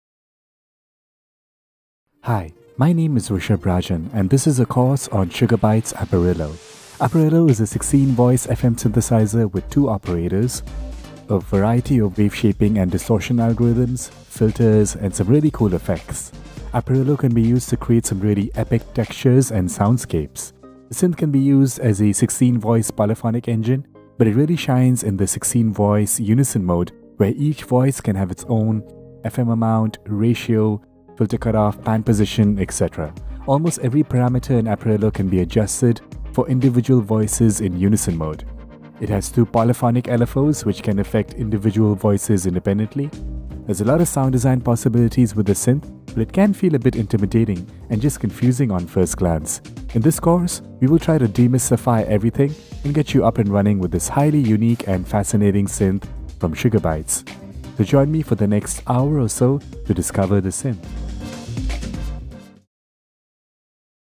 Sugar Bytes 101: Aparillo Sound Design - 1. Introduction
Sugar Bytes 101: Aparillo Sound Design by Rishabh Rajan
Video 1 of 23 for Sugar Bytes 101: Aparillo Sound Design

Sugar Bytes Aparillo is based on FM synthesis, but its definitely not your standard digital synth plugin. A quick look at the user interface may leave you confused and when you switch to the Orbit tab, you will probably feel like you've been teleported at the control board of an alien spaceship. Thankfully, sound designer Rishab Rajan is here to demystify everything about this very special instrument!

Rishabh starts the course with a quick overview of the synth. After explaining where everything is found on the interface, he dissects all the knobs, controls and panels. You learn how every parameter (Ratio, FM Index, Formant/Waveshaping, Jitter, LFOs, Envelopes, etc.) work together to create complex timbres and organic textures, all while strengthening your knowledge of FM synthesis. You also explore Aparillos arpeggiator (which behaves in very unique ways) and you get an in-depth look at the effect section (which includes some very unusual and great sounding filters). Rishabh ends the course by covering everything about the fascinating Orbit tab...

So buckle your seat, watch this 23-tutorial course and get ready to rocket into Orbit with Sugar Bytes Aparillo!